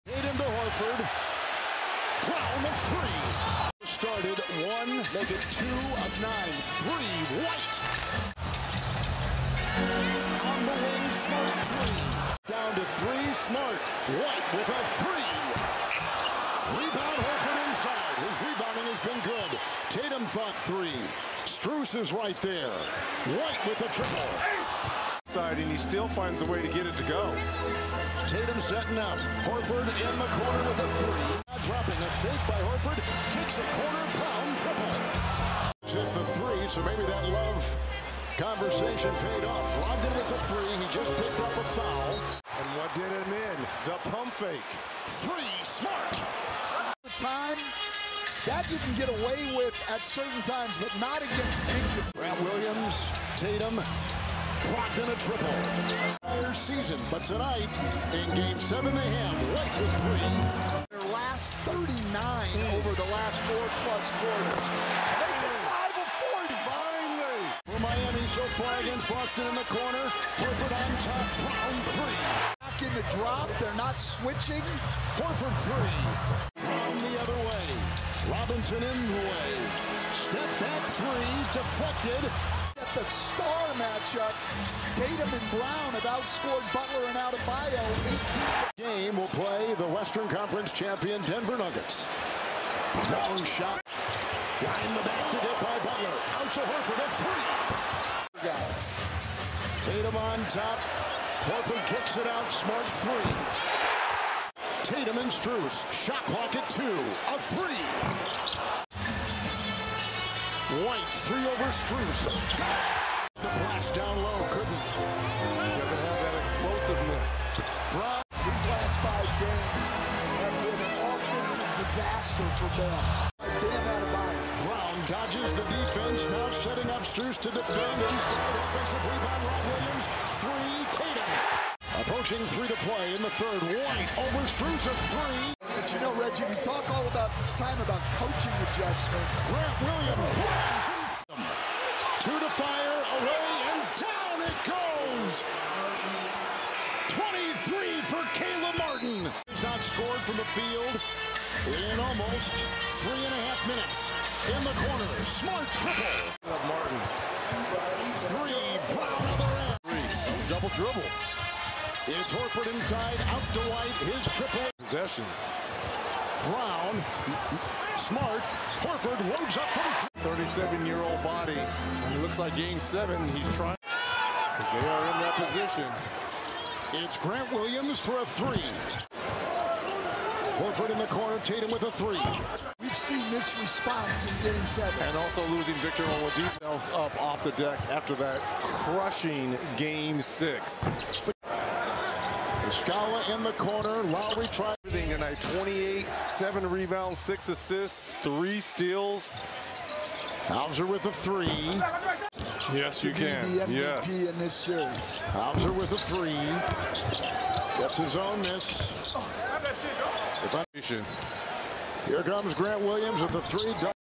All the 3-pointers the Boston Celtics players took in Game 7 vs. the Miami Heat 9/42 (21.4%)
What a bad game for them.

Boston Celtics vs. Miami Heat - Game 7 - 5/29/23

2023 NBA Playoffs